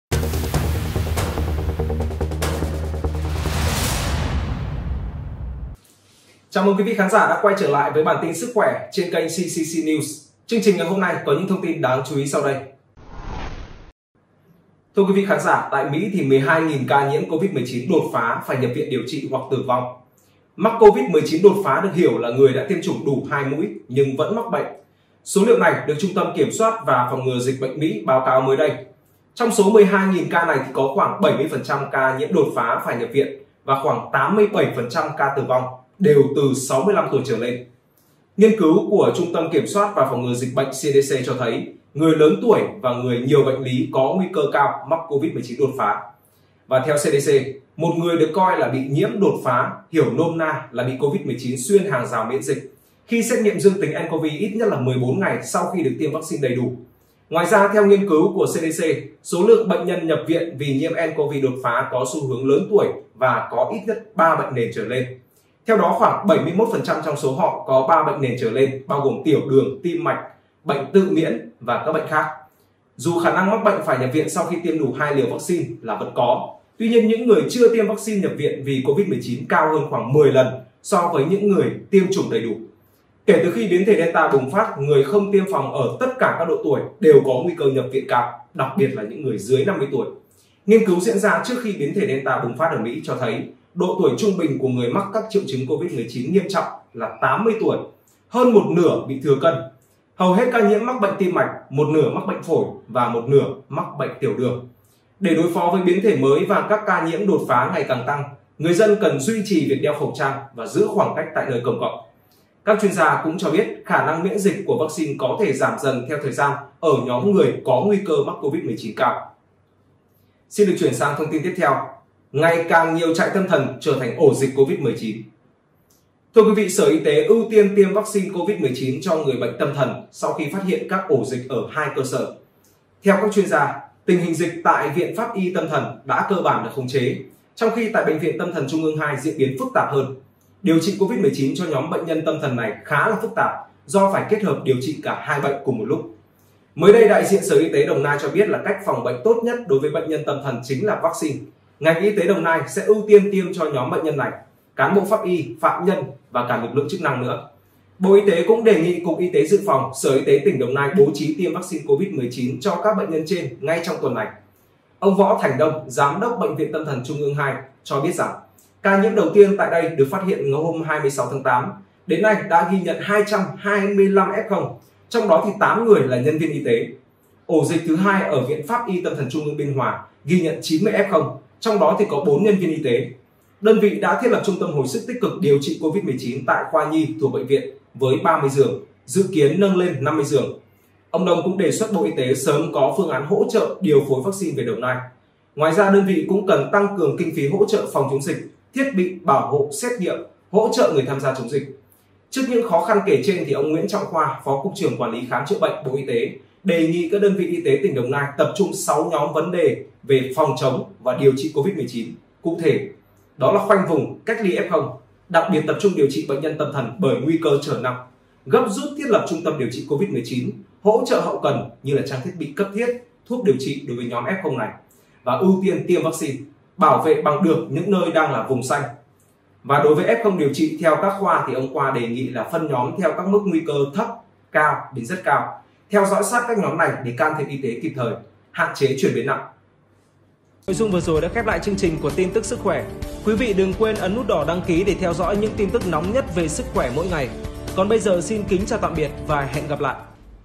🛑12.000 Người Mắc Bệnh Dù Đã Tiêm Đủ 2 mũi
Chào mừng quý vị đã đến với kênh “Tin Tức Sức Khỏe” Chương trình hôm nay gồm những tin chính sau:
- Tại Mỹ, 12.000 là F0 dù đã tiêm đủ 2 mũi
- Ngày càng nhiều trại tâm thần trở thành ổ dịch Covid-19